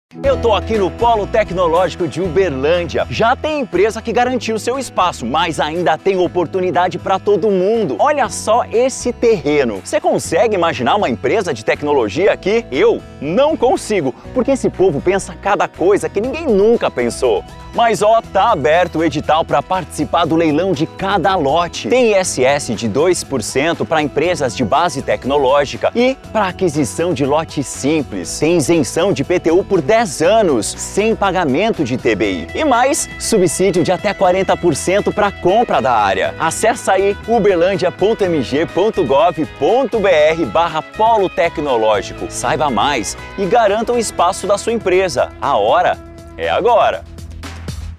O local ideal para sua empresa de base tecnológica!
Inovação | Já imaginou o seu negócio isento de IPTU por uma década? Estes e muitos outros benefícios são exclusivos para empresas que adquirirem um lote no Polo Tecnológico Sul. Confira mais detalhes clicando no link dos stories.